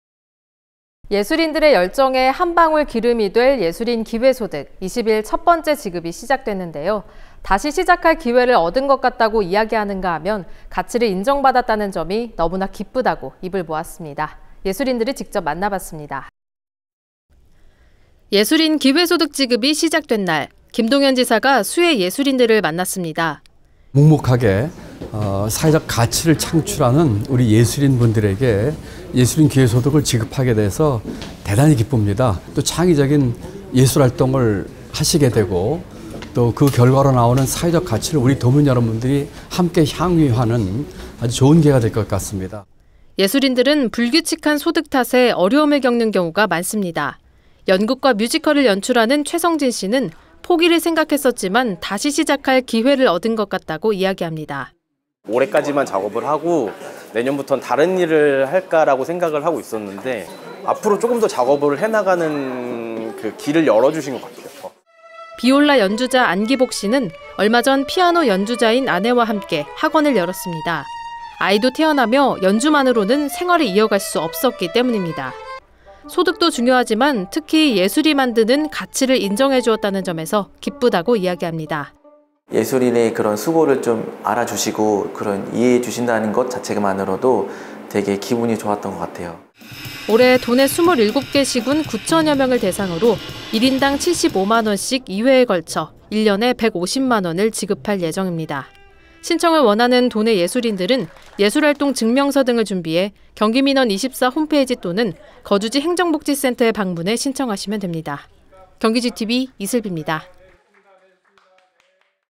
예술 열정 태울 ‘예술인 기회소득’ 첫 지급 시작
[앵커멘트]
예술인들의 열정에 한 방울 기름이 될 예술인 기회소득.

20일 첫 번째 지급이 시작됐는데요.

다시 시작할 기회를 얻은 것 같다고 이야기하는가 하면, 가치를 인정 받았다는 점이 너무나 기쁘다고 입을 모았습니다.

예술인들을 직접 만나 봤습니다.

[리포트]

예술인 기회소득 지급이 시작된 날.

김동연 지사가 수혜 예술인들을 만났습니다.

묵묵히 사회적 가치를 창출하는 예술인들에게 예술인 기회소득을 지급하게 되어 기쁩니다. 창의적인 예술 활동을 하시게 되고 그 결과로 나온 사회적 가치를 도민 여러분들이 함께 향유하는 좋은 기회가 될 것 같습니다.

예술인들은 불규칙한 소득 탓에 어려움을 겪는 경우가 많습니다.

연극과 뮤지컬을 연출하는 최성진씨는 포기를 생각했었지만, 다시 시작할 기회를 얻은 것 같다고 이야기합니다.

[인터뷰] 최성진 / 연극·뮤지컬 연출자
올해까지만 작업을 하고 내년부터 다른 일을 할까 생각하고 있었는데 앞으로 조금 더 작업을 해나가는 길을 열어주신 것 같아요.

비올라 연주자 안기복씨는 얼마 전 피아노 연주자인 아내와 함께 학원을 열었습니다.

아이도 태어나며 연주만으로는 생활을 이어갈 수 없었기 때문입니다.

소득도 중요하지만 특히 예술이 만드는 가치를 인정해 주었다는 점에서 기쁘다고 이야기합니다.

[인터뷰] 안기복 / 비올라 연주자
예술인의 수고를 알아주시고 이해해주신다는 것만으로도 기분이 좋았습니다.

올해 도내 27개 시군 9천여 명을 대상으로 1인당 75만 원씩 2회에 걸쳐 1년에 150만 원을 지급할 예정입니다.

신청을 원하는 도내 예술인들은 예술활동증명서 등을 준비해 경기민원24 홈페이지 또는 거주지 행정복지센터에 방문해 신청하시면 됩니다.

[자막]

1. 예술인 기회소득 수혜자 간담회(7월 20일)

2. [현장음] 김동연 / 경기도지사

3. 생계 어려워 포기하려는 경우 많아

4. [인터뷰] 최성진 / 연극·뮤지컬 연출자

5. “예술의 가치 인정받았다는 점에서 기뻐”

6. [인터뷰] 안기복 / 비올라 연주자

7. 올해 9천여 명 대상 지급 예정

8. 박스자막

9. 영상촬영: 이민준 영상편집: 윤지성 CG: 우윤정